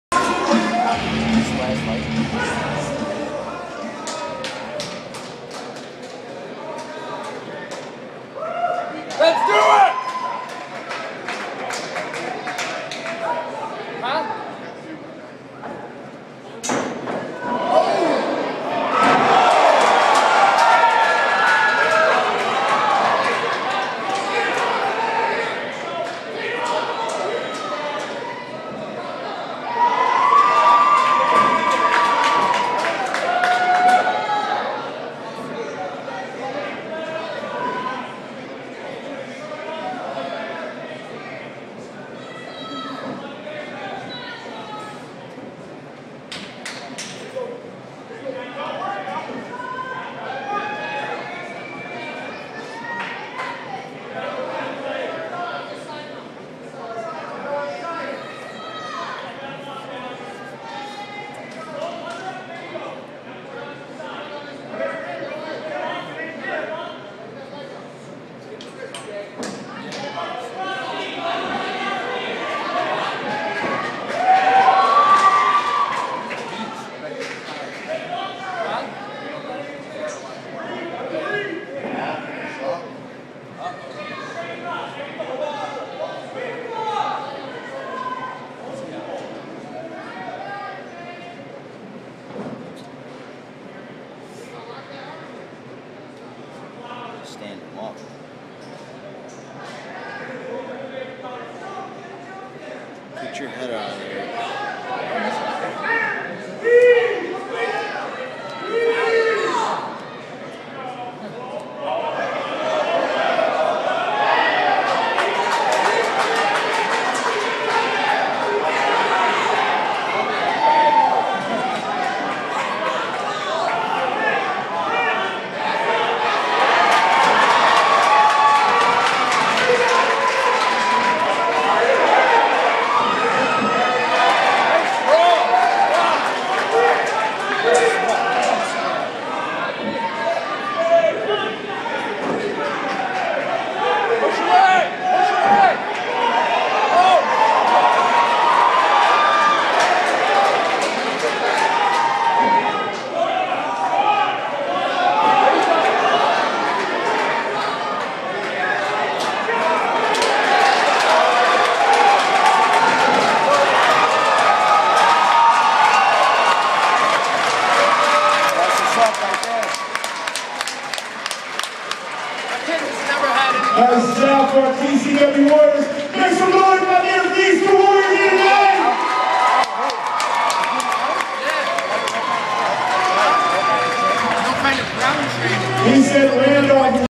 Cage fights in Olean High School
Cage Fight in Olean, New York in the High School Gym... Kid trys a striAGHT kick and gets SLAMMED!